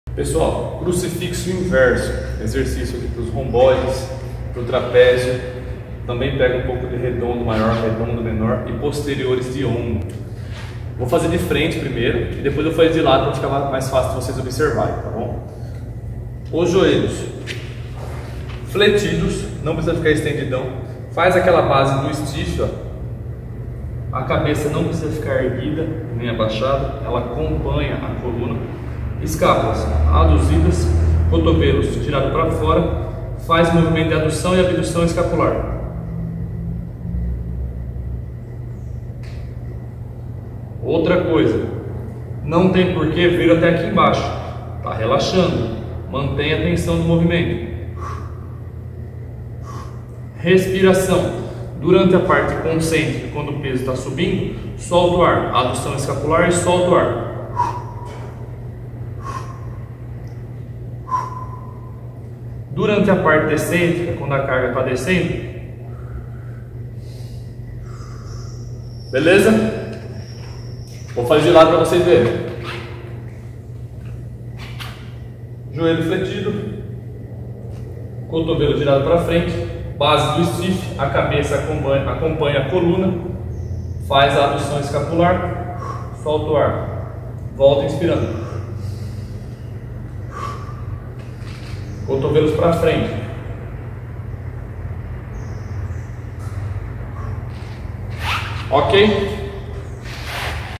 Como fazer crucifixo inverso
Assista ao vídeo e aprenda a executar o exercício crucifixo inverso de forma correta.

Vem comigo!